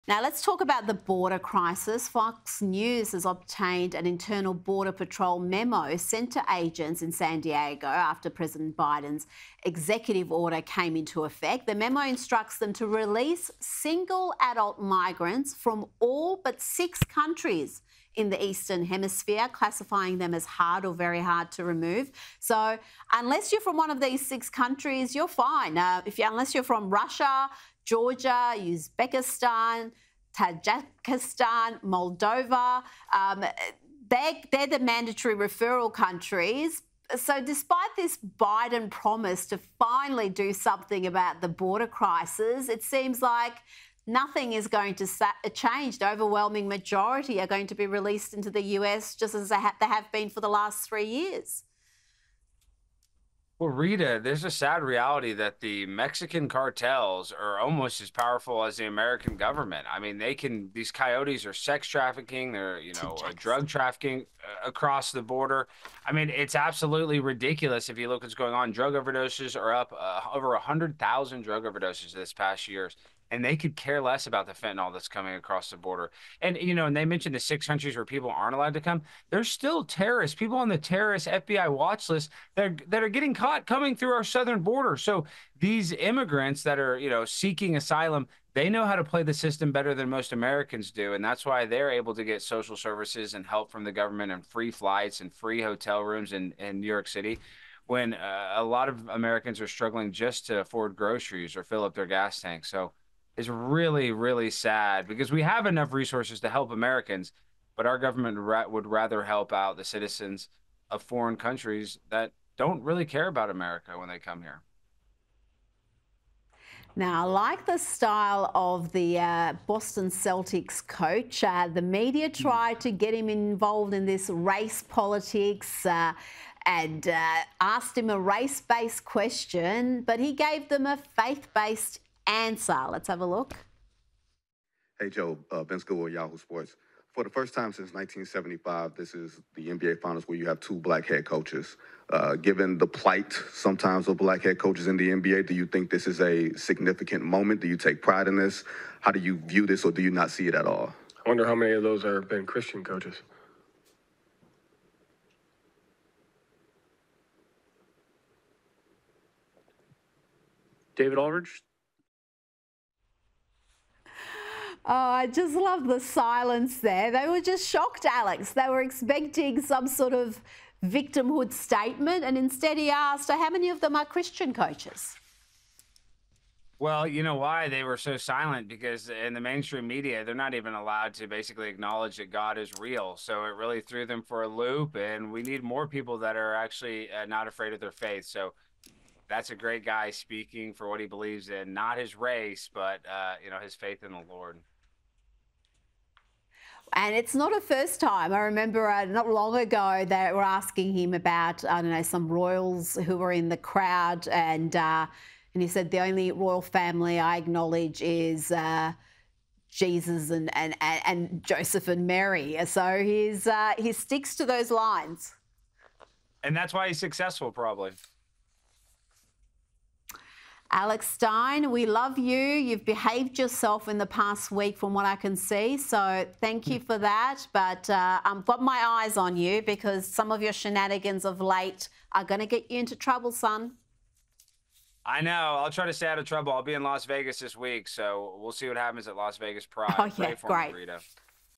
US government ‘would rather’ help out immigrants than American citizens
Comedian Alex Stein says the US government is more interested in helping out immigrants than its own American citizens.

“These immigrants that are seeking asylum – they know how to play the system better than most Americans do,” Mr Stein told Sky News host Rita Panahi.

“That’s why they’re able to get social services and help from the government and free flights and free hotel rooms in New York City when a lot of Americans are struggling just to afford groceries or fill up their gas tank.

“It’s really, really sad because we have enough resources to help Americans, but our government would rather help out the citizens of foreign countries that don’t really care about America when they come here.”